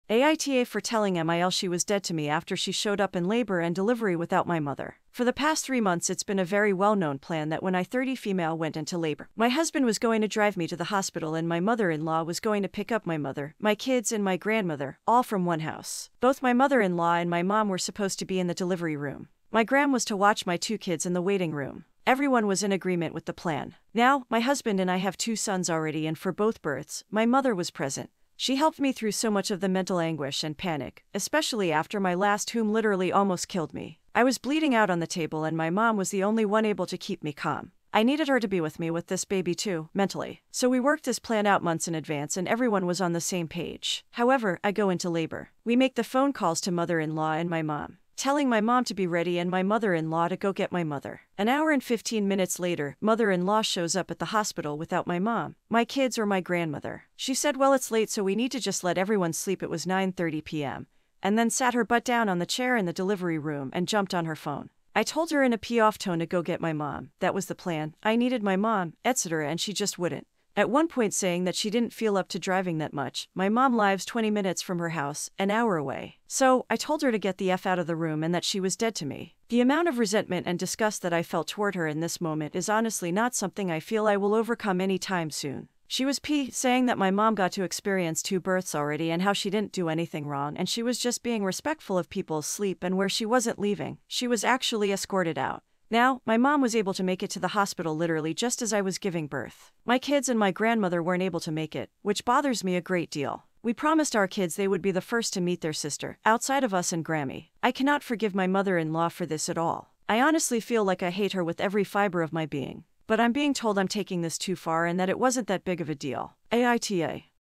My MIL showed up in labor and delivery without my mother | Best of AITA Reddit Stories | Full Story
AITA for telling MIL she was dead to me after she showed up in labor and delivery without my mother?

I'm not going to my sister's wedding.